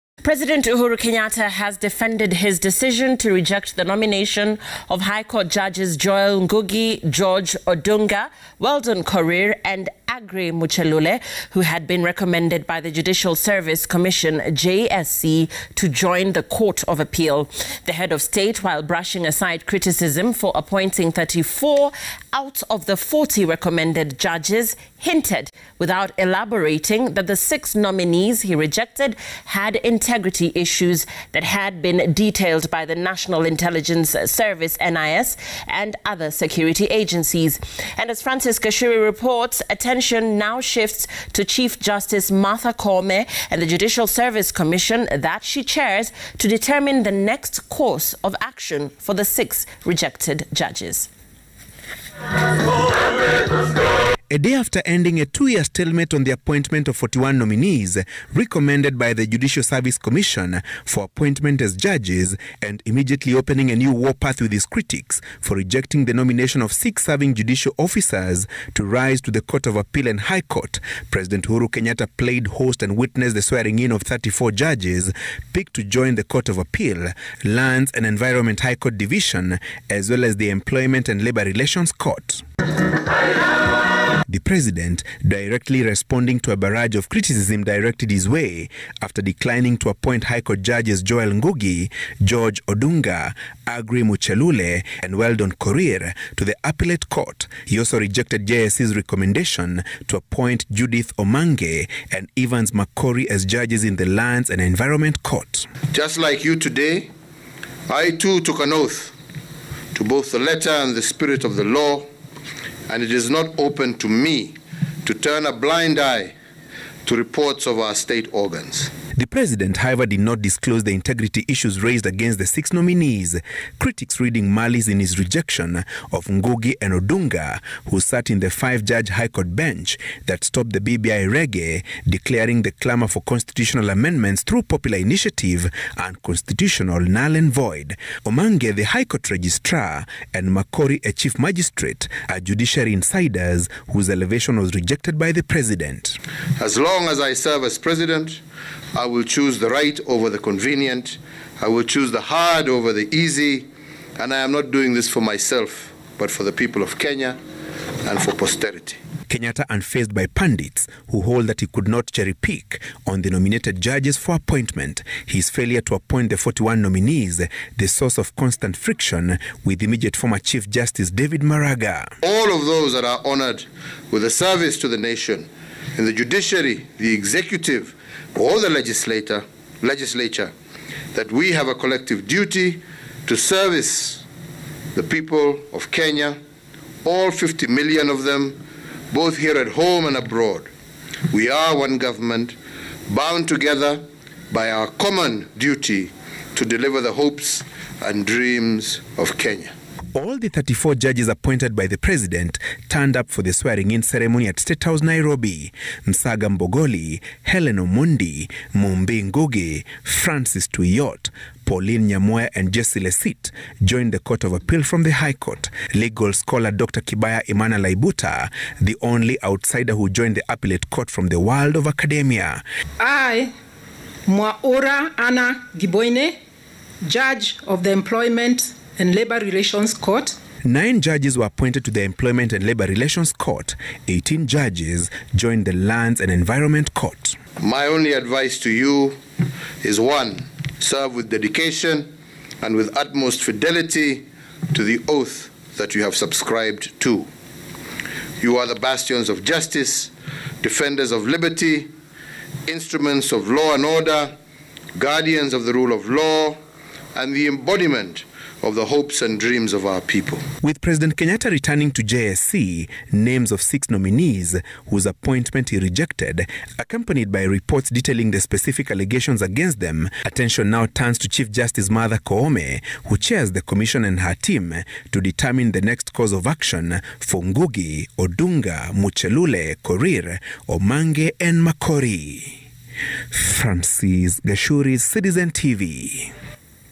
Pres Kenyatta defends decision to reject 6 judges
Uhuru says decision based on reports from state organs
Uhuru: I can’t turn blind eye to reports of state organs
Uhuru: As President, I’ll choose the hard over the easy
Uhuru presides swearing in of 34 judges